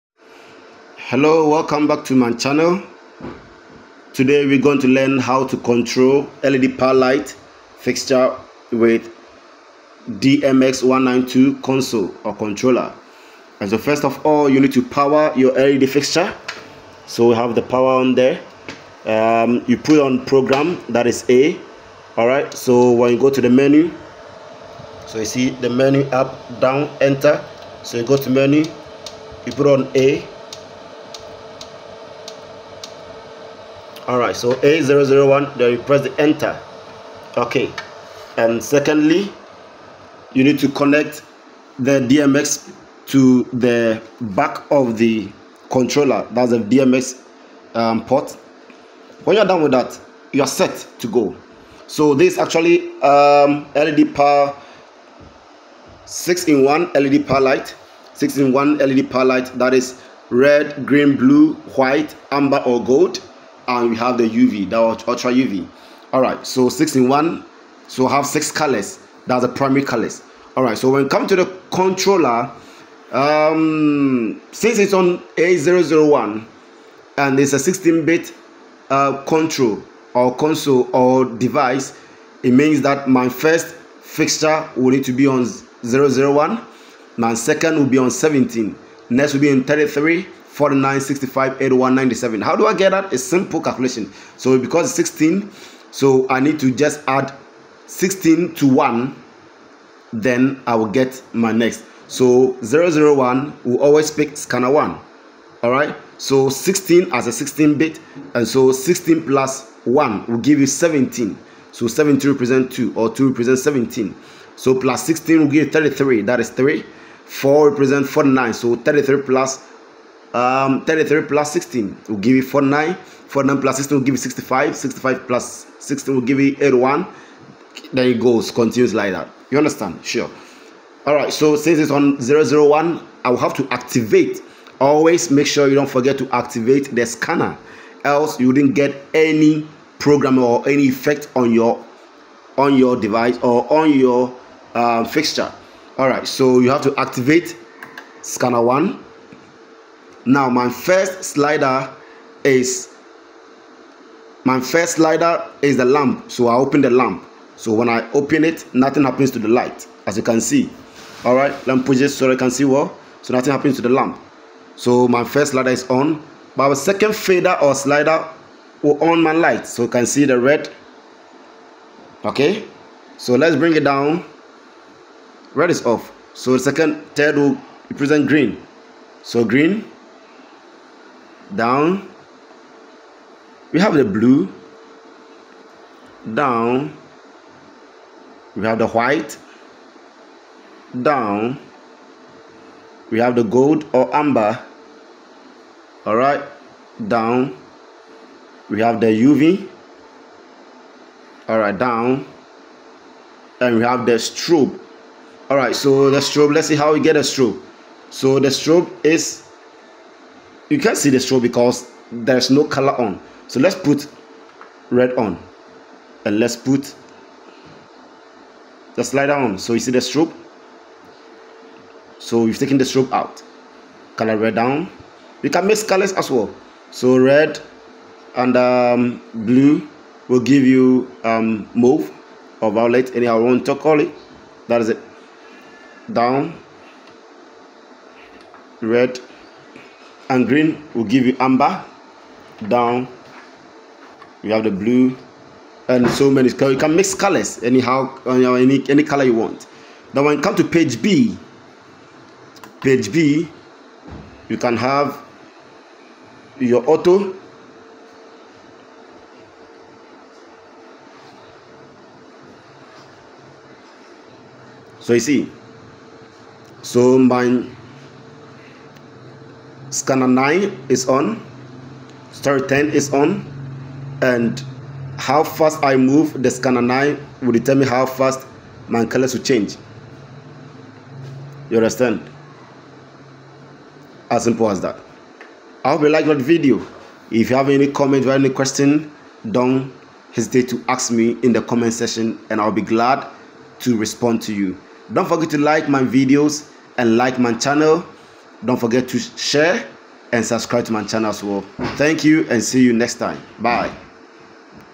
HOW TO CONTROL LED PAR LIGHT WITH DMX CONTROLLER 192
This video will show you how to control your led par light fixture with the 192 DMX Controller.